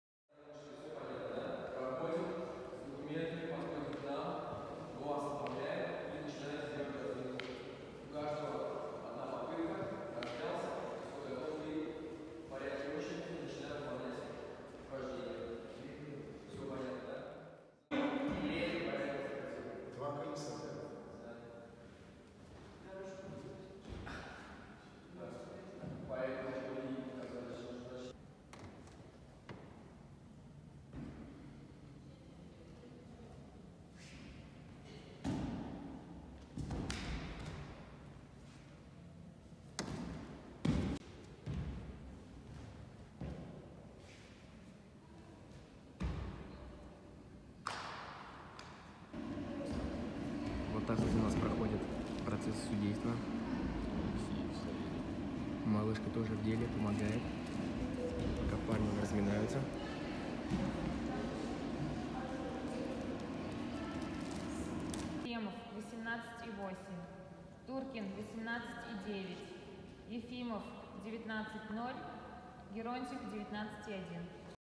Мы в качестве судей, на Всероссийской Олимпиаде школьников по физической культуре
Уже шестой год с @yulyalya4 в судейской коллегии 🎓😎☝🏻
 На муниципальном этапе Всероссийской Олимпиаде школьников по физической культуре 🏆🥇
И в этом году, наша Малышка помогала нам оценивать мастерство ребят 👶🏻 📝🤸🏼‍♂️
Приятно видеть, что с каждым годом становиться все больше участников Олимпиады 💪🏻😉
Больше фото и видео у меня в истаграме: